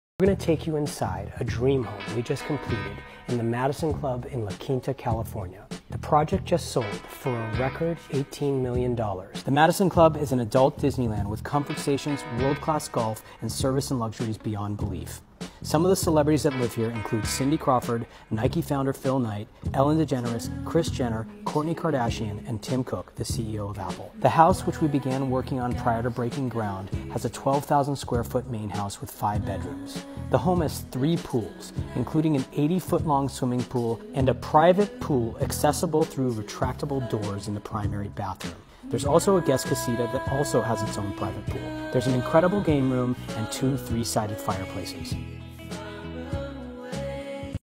[EXCLUSIVE] Record-Breaking $18 Million Dollar Home
Behind the scenes of our work in the Madison Club, as featured in the Wall Street Journal. This project led to a record-breaking sale of $18 million after less than 2 weeks on the market.